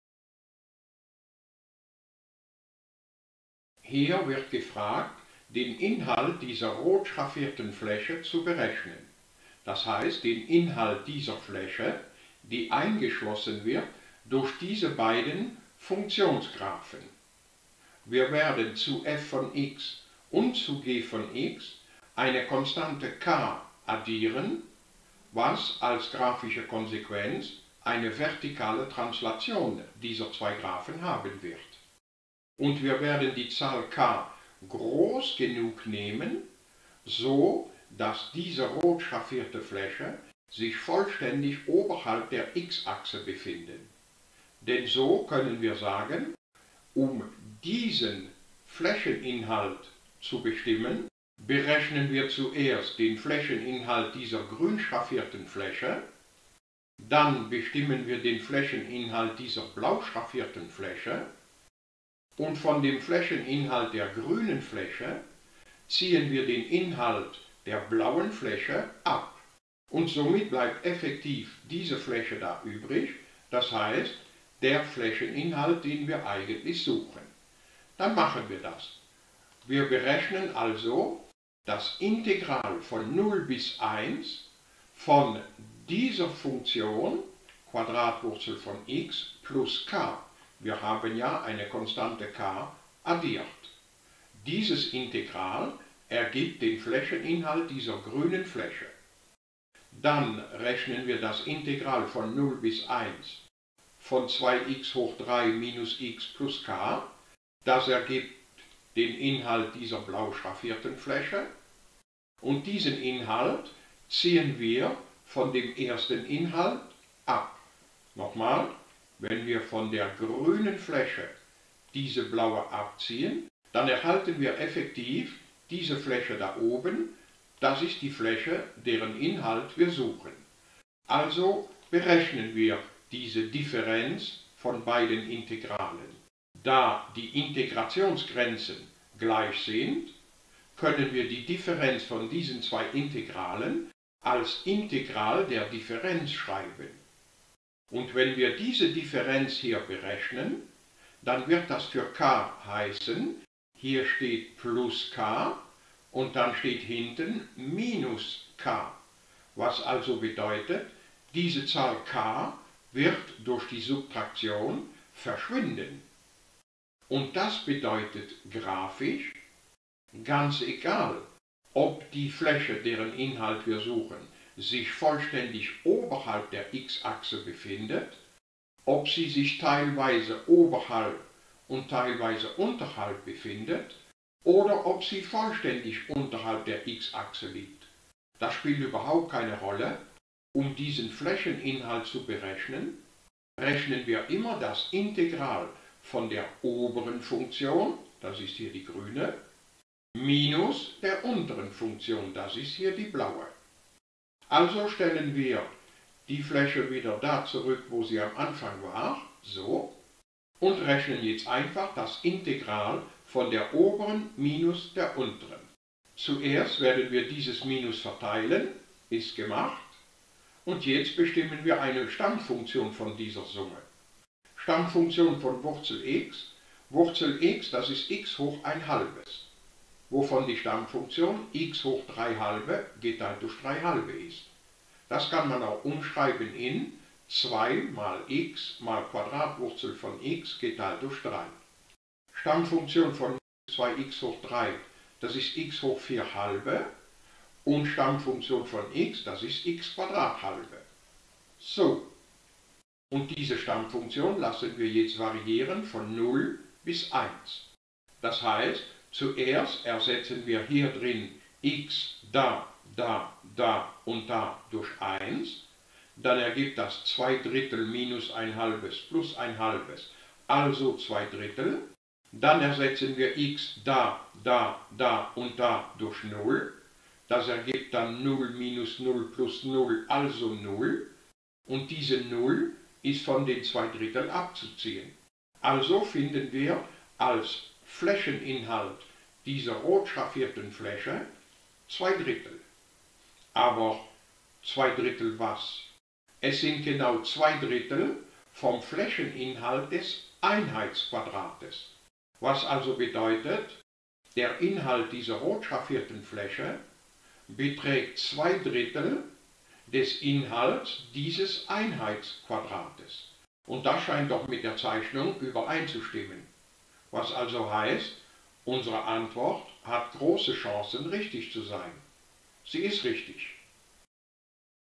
Integrale 009
Wir berechnen hier als Integral den Inhalt einer Fläche, die durch zwei Funktionsgraphen eingeschlossen wird.